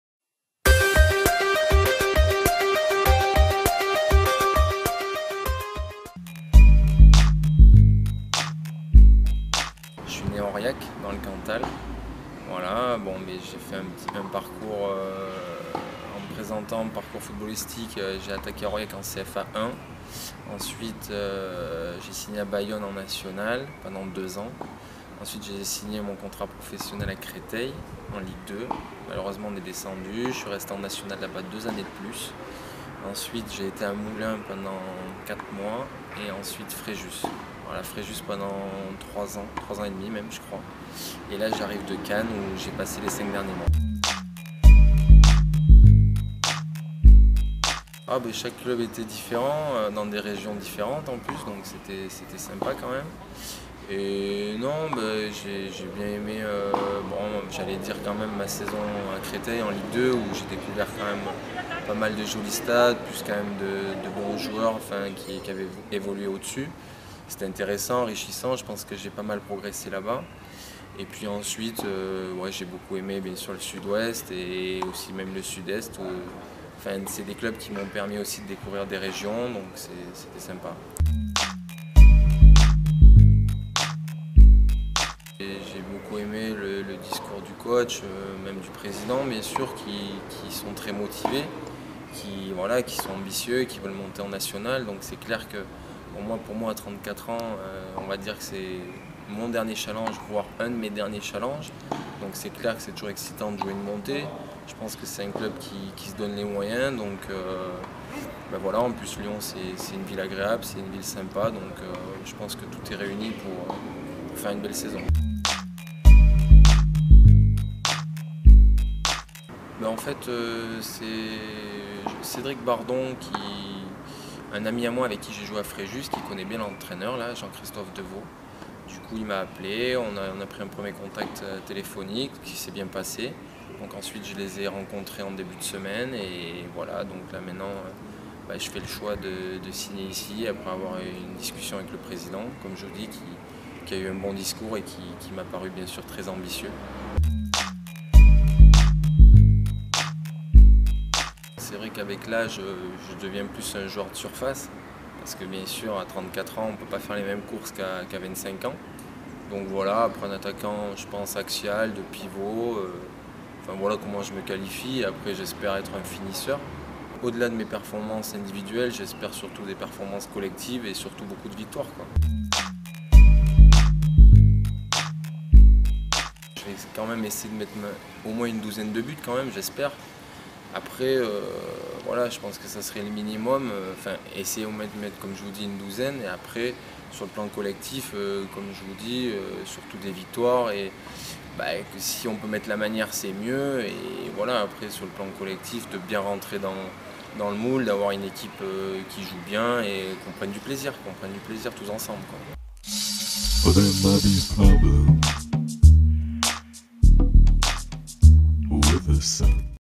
Patrice Vareilles : "J'ai beaucoup aimé le discours du coach et du président"
L'interview de Patrice Vareilles, nouvelle recrue de Lyon Duchère AS.